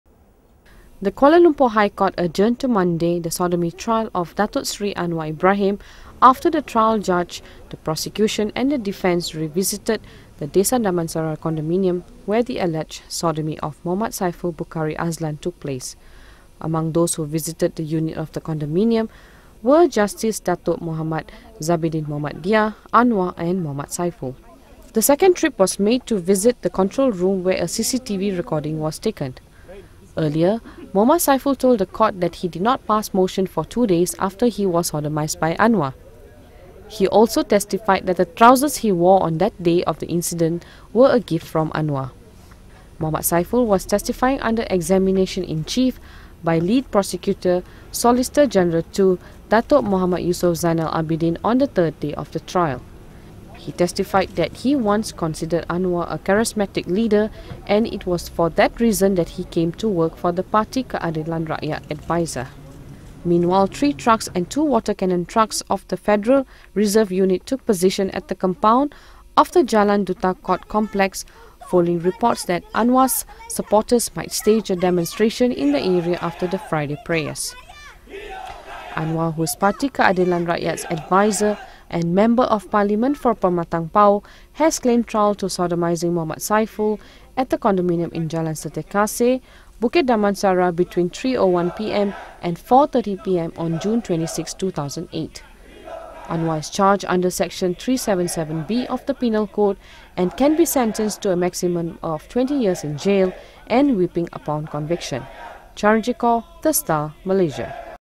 Sodomy II - Day 3
The sodomy trial of Datuk Seri Anwar Ibrahim adjourns to Monday. Mohd Saiful Bukhari Azlan, the first witness to take the stand, completes his examination-in-chief Friday.